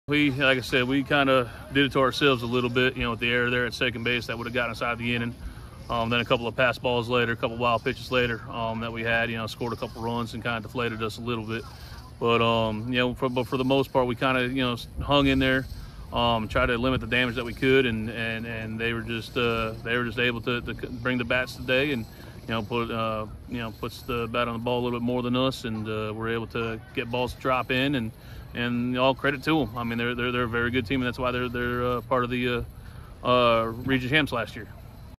WATCH NOW: Darlington Baseball Coach Joe Kolodziej
DARLINGTON, S.C. -- Darlington baseball coach Joe Kolodziej talks about his team's 12-0 loss to South Florence on Tuesday.